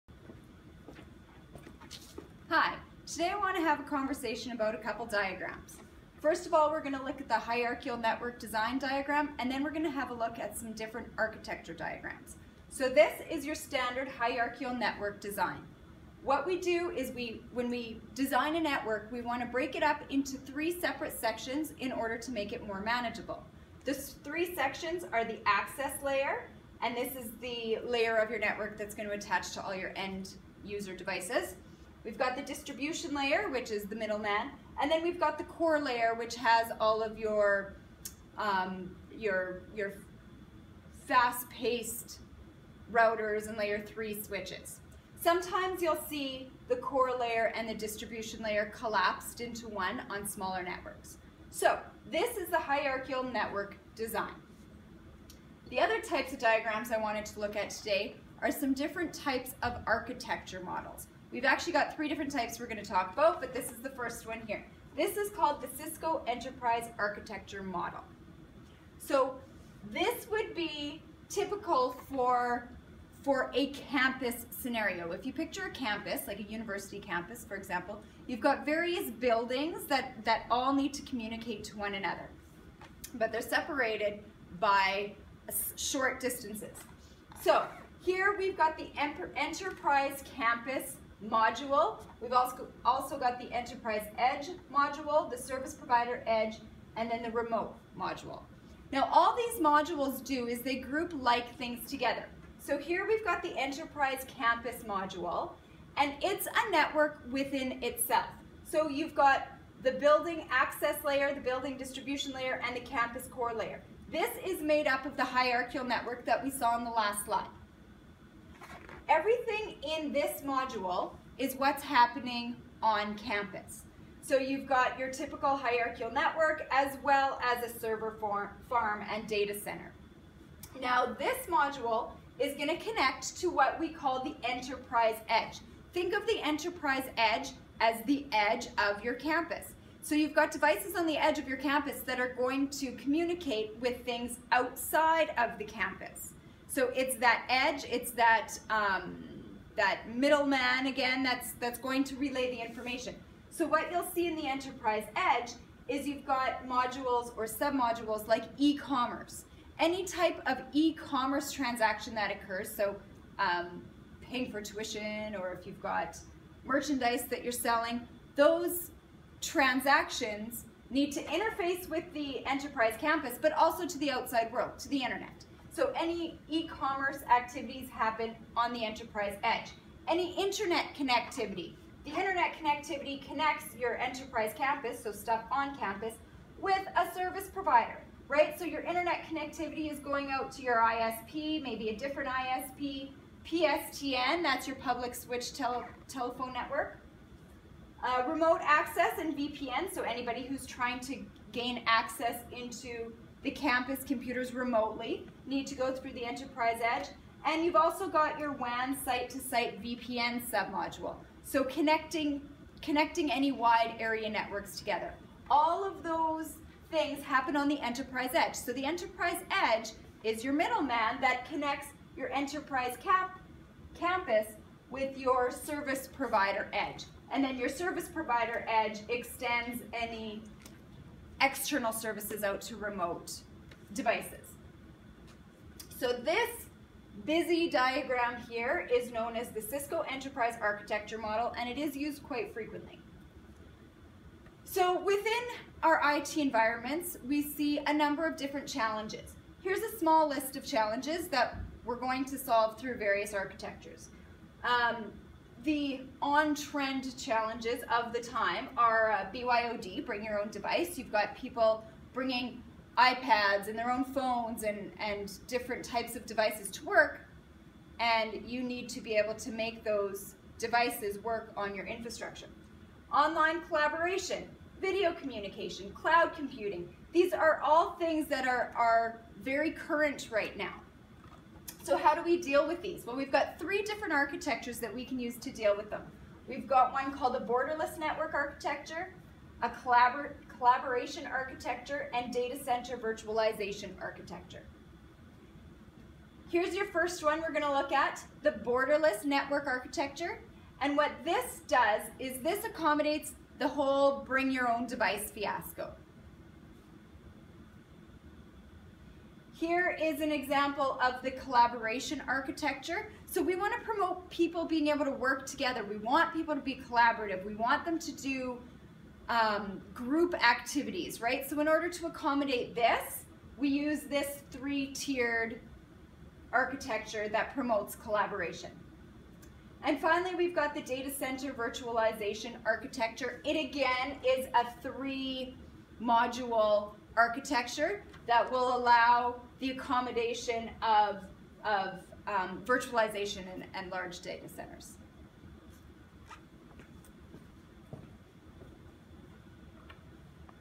Hierarchical Network Design
A brief introduction to the hierarchical network design, Enterprise Architecture model with enterprise campus and enterprise edge.  Also solving tending IT challenges with the Borderless Network Architecture, Collaboration Architecture and Data Center/Virtualization Architecture.,